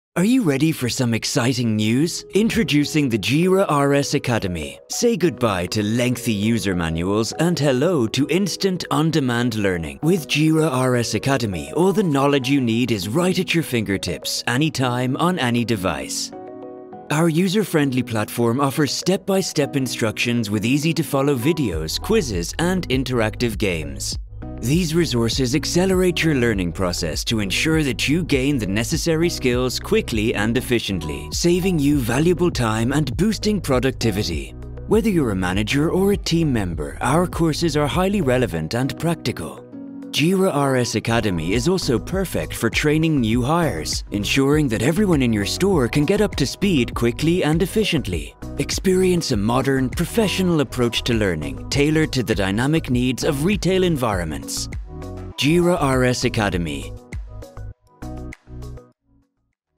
GERA RS Academy
Are you ready for some exciting news? Introducing the Gera RS Academy!

Say goodbye to lengthy user manuals and hello to instant, on-demand learning. With Gera RS Academy, all the knowledge you need is right at your fingertips, anytime, on any device.

Our user-friendly platform offers step-by-step instructions with easy-to-follow videos, quizzes, and interactive games. These resources accelerate your learning process to ensure that you gain the necessary skills quickly and efficiently, saving you valuable time and boosting productivity.

Whether you're a manager or a team member, our courses are highly relevant and practical. Gera RS Academy is also perfect for training new hires, ensuring that everyone in your store can get up to speed quickly and efficiently.

Experience a modern, professional approach to learning, tailored to the dynamic needs of retail environments.

Gera RS Academy – Your Knowledge, Your Success.